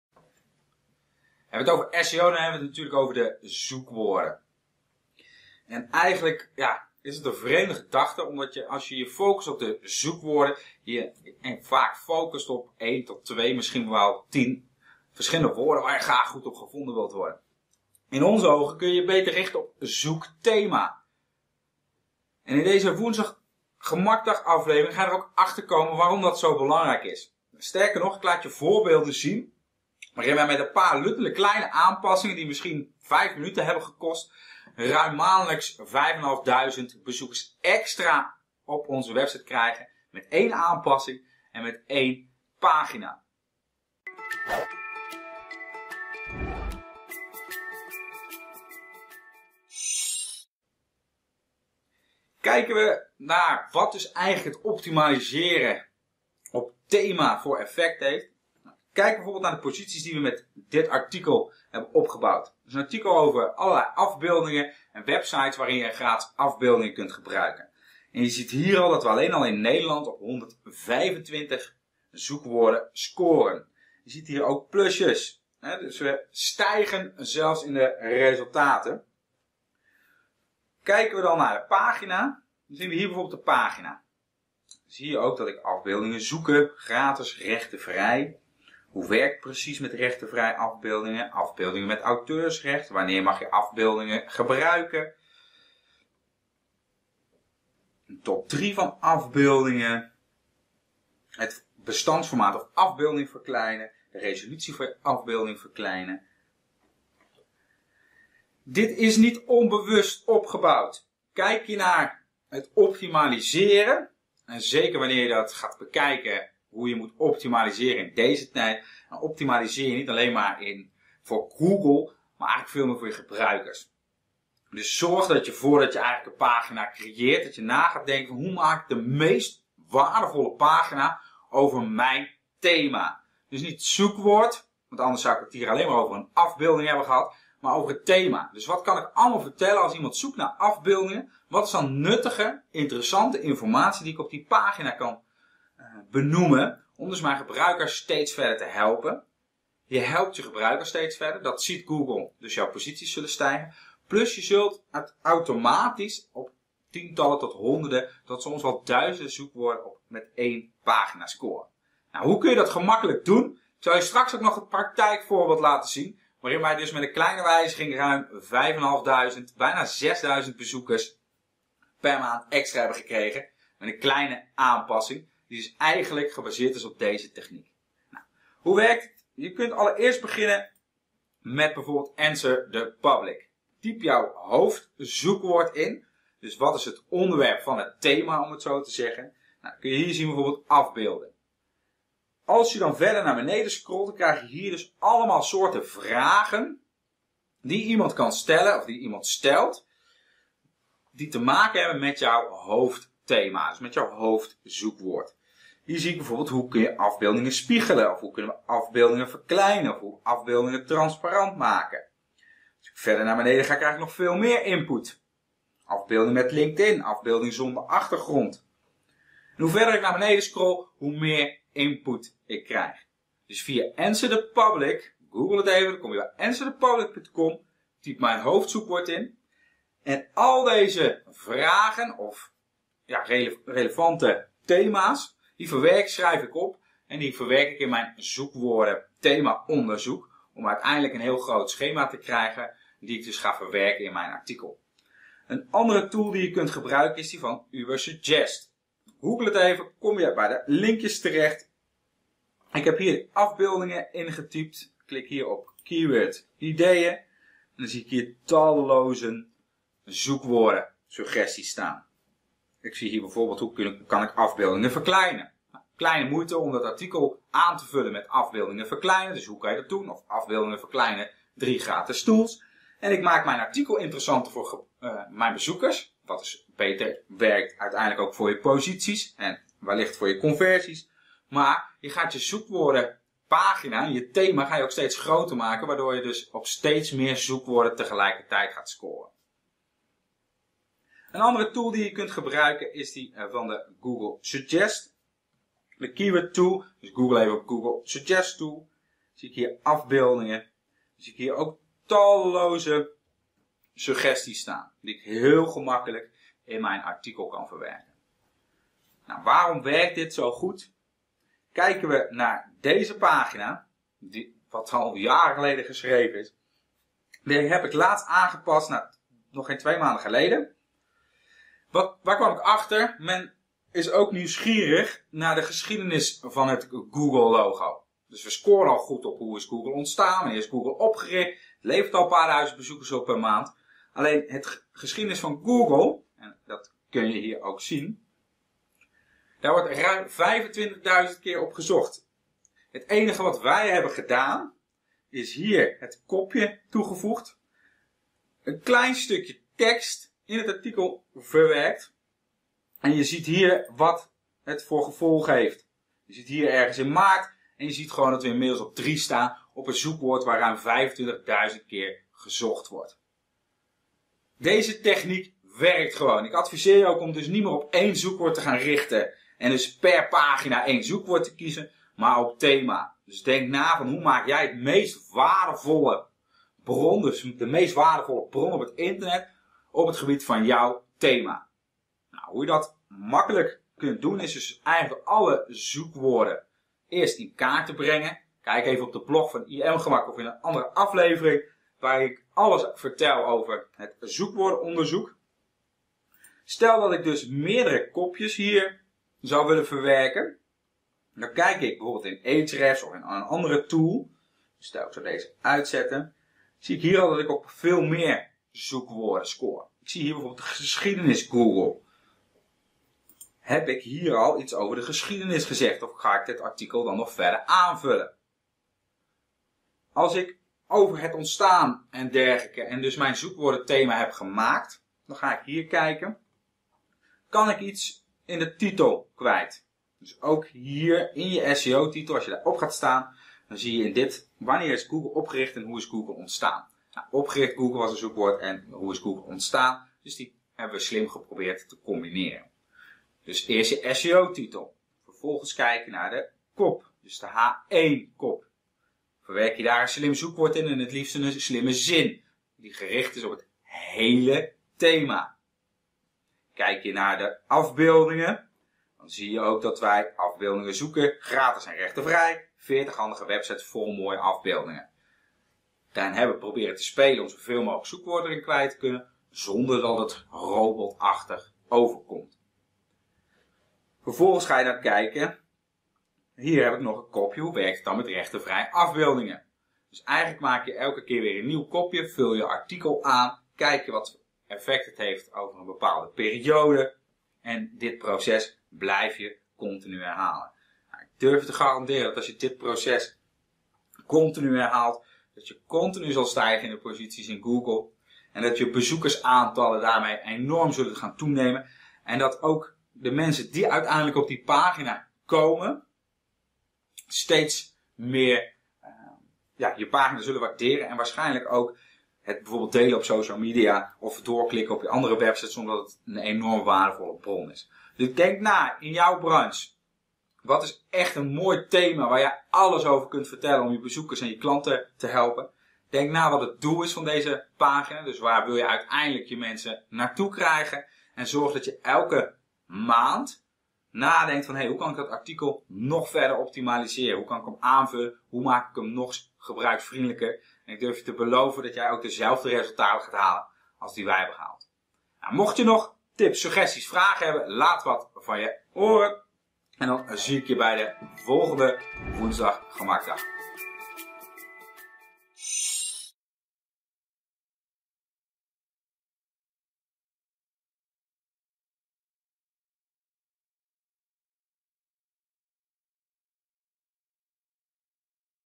Hoger in Google met SEO: 1 pagina, 126 posities in Google!
Hoger in Google Komen? Gun je website meer bezoekers, zonder het harde werk.

Ontdek de zoekmachine marketingstrategie om op honderden zoekwoorden te scoren.

Met slechts 1 pagina.

 👉 𝐃𝐞 𝟑 𝐒𝐄𝐎 𝐇𝐚𝐜𝐤𝐬 𝐯𝐨𝐨𝐫 𝟏𝟎.𝟎𝟎𝟎 𝐛𝐞𝐳𝐨𝐞𝐤𝐞𝐫𝐬 (𝐢𝐧 𝟗𝟎 𝐝𝐚𝐠𝐞𝐧)

Bekijk de video en ontdek:

🚀 Hoe een eenvoudige aanpassing van 5 minuten ons meer dan 5500 extra websitebezoekers per maand opleverde.

🔍 Welke SEO-tips ervoor zorgen dat jouw pagina op meer dan 100 zoekwoorden tegelijkertijd wordt gevonden.

🛠️ Welke tools je kunnen helpen bij het slim opbouwen van je SEO-contentstrategie.